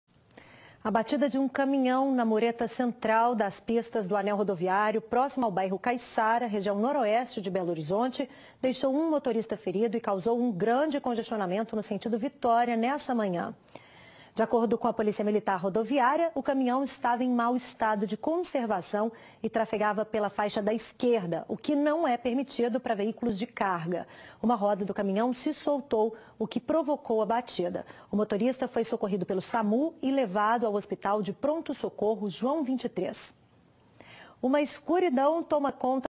Caminhão perde roda e bate em mureta do Anel Rodoviário
A batida provocou um grande congestionamento na rodovia e o motorista do veículo ferido.